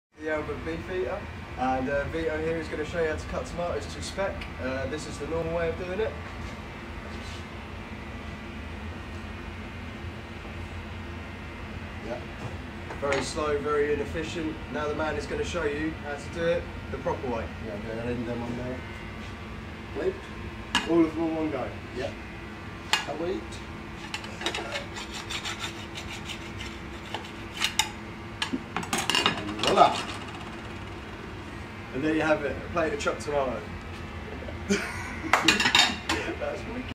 Chefs masterclass-tomato slicing
How to increase efficiency in the kitchen